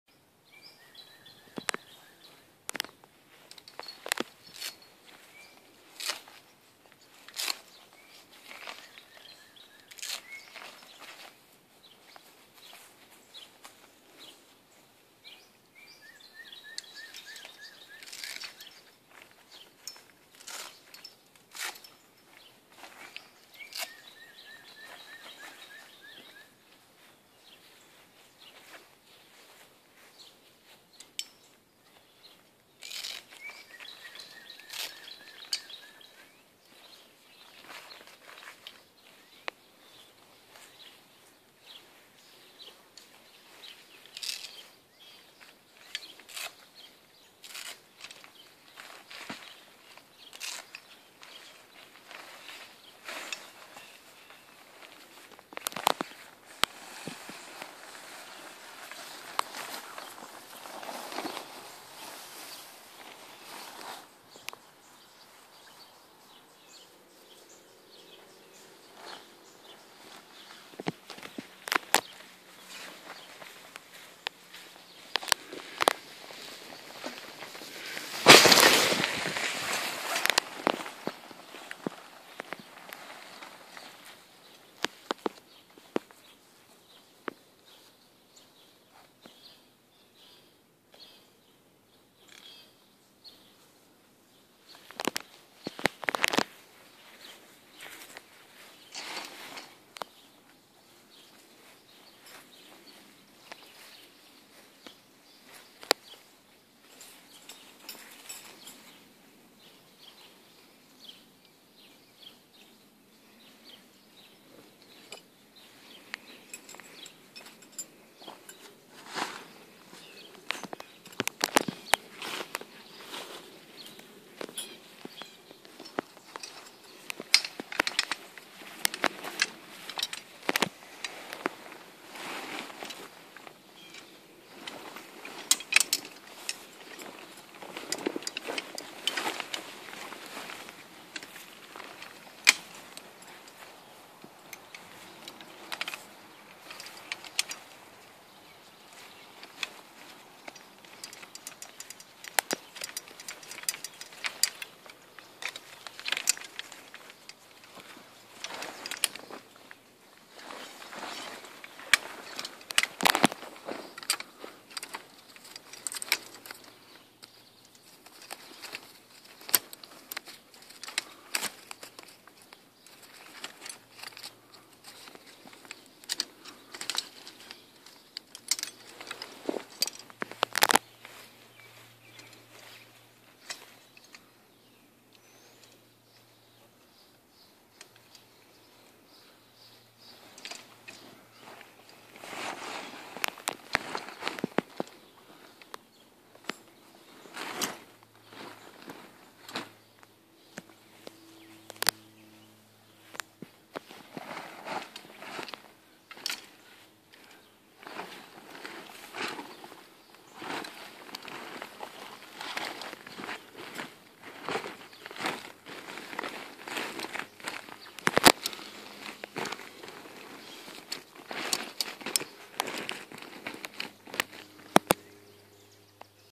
Taking Down the Jet Tent F25
The Jet Tent series offers the ability to set up or take down quickly! The release of the center hub allows the legs to unlock and pack away for easy storage.. Find more